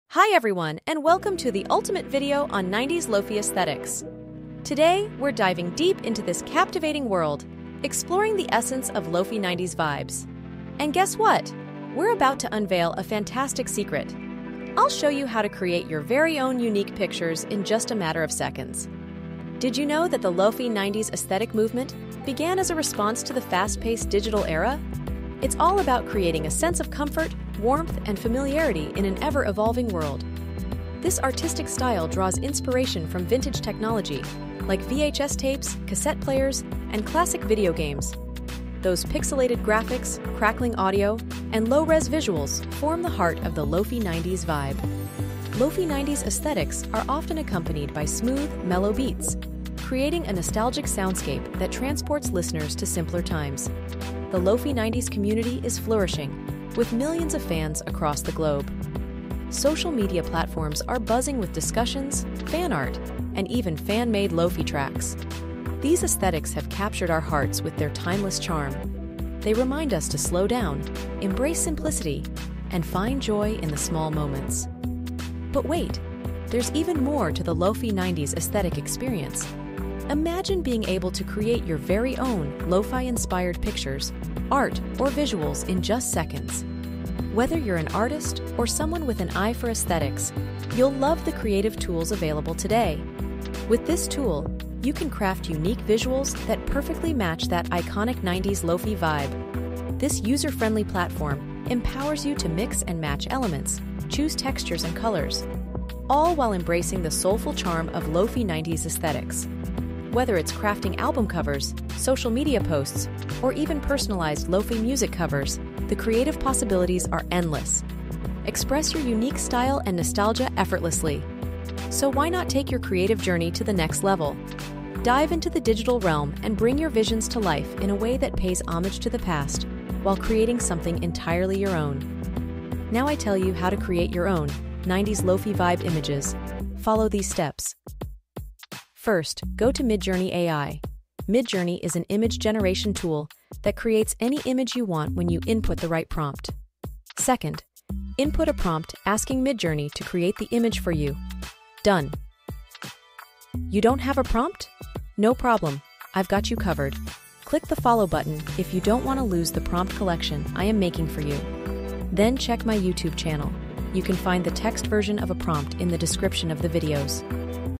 Images in Lofi 90s style How to Draw Image MidJourney AI Prompt
Use this prompt in MidJourney to create images in Lofi 90s style:
Prompt Template:
[type of image] [scene description] [--aspect ratio] [--MidJourney model] [--stylize]

Explanation:
[type of image] - Specify the category or style of the image. Comic-style, bold, neon colors.
[scene description] - Provide a vivid depiction of the scene or scenario.
[--aspect ratio] - Defines the width-to-height ratio of the image. 16:9 for horizontal images.

Prompt example:
Perspective Side view of the writing desk Details Young man writing a letter at a desk by a window the natural light